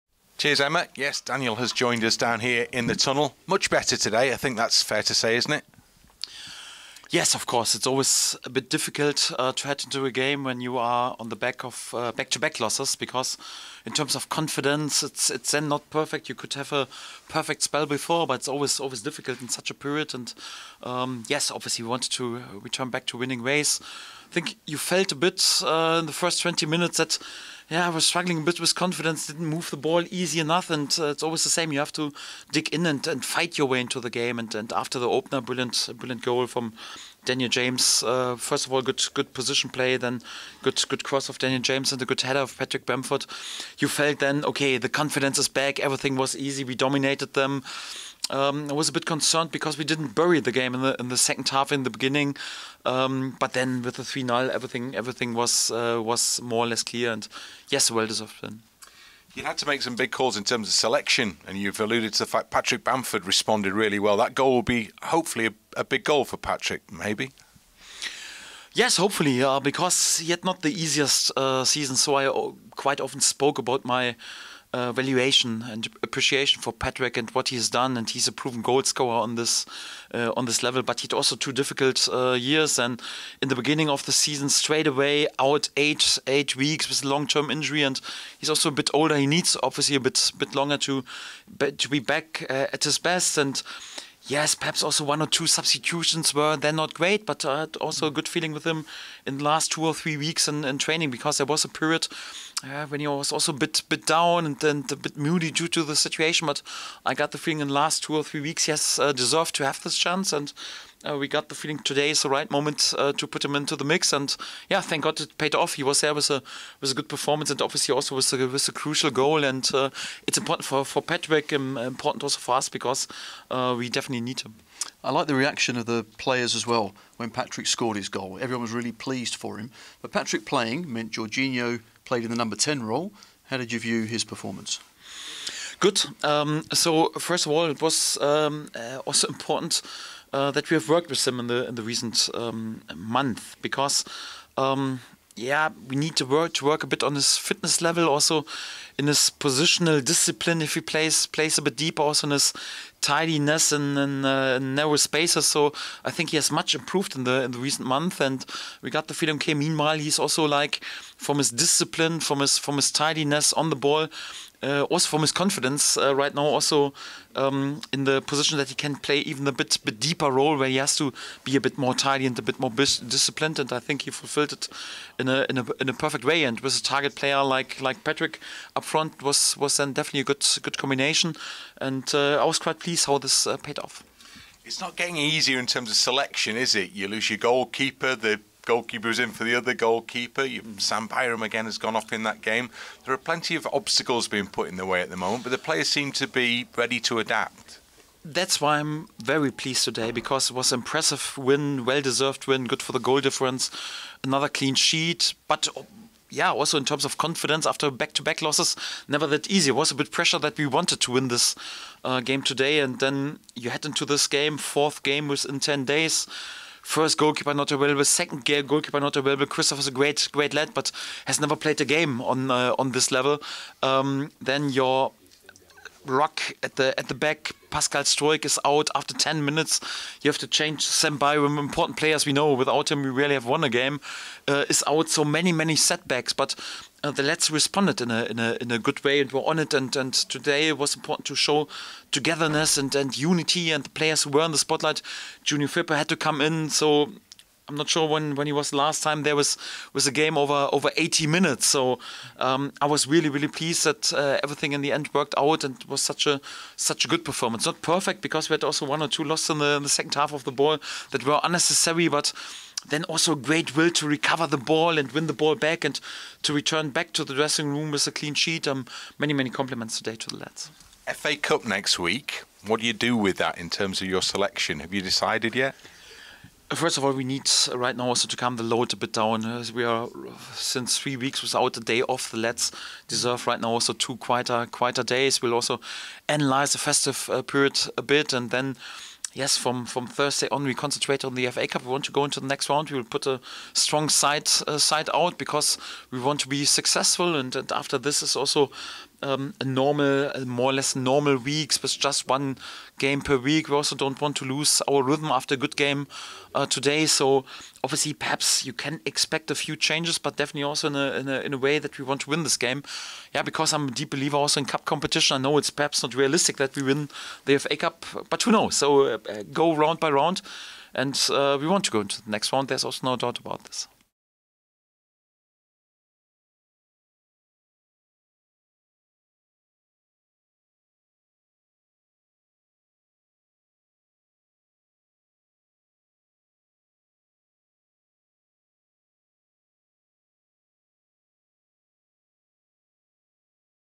“A well deserved win” | Daniel Farke reaction | Leeds United 3-0 Birmingham City
Manager Daniel Farke gives his reaction to our 3-0 win over Birmingham City at Elland Road in the EFL Championship.